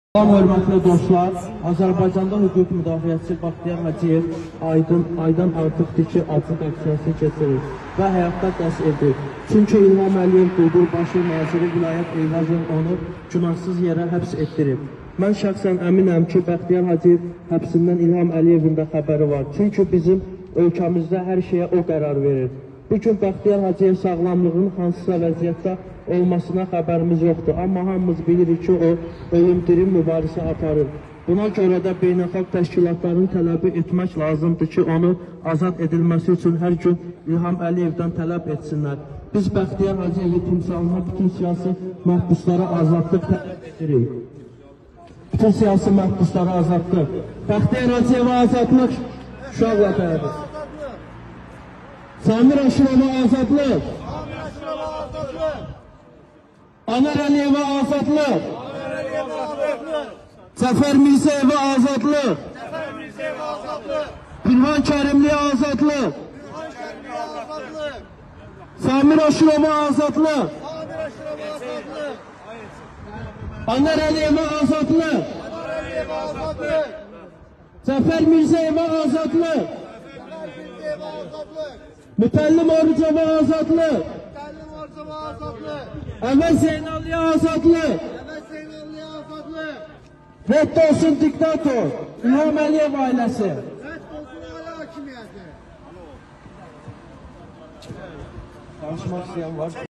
DAS və Almaniyada yaşayan fəallar, siyasi məhbuslara və Bəxtiyar Haciyevə azadlıq tələb edirlər. 2
DAS organization and activists living in Germany demand the freedom of political prisoners and Bakhtiyar Hajiyev.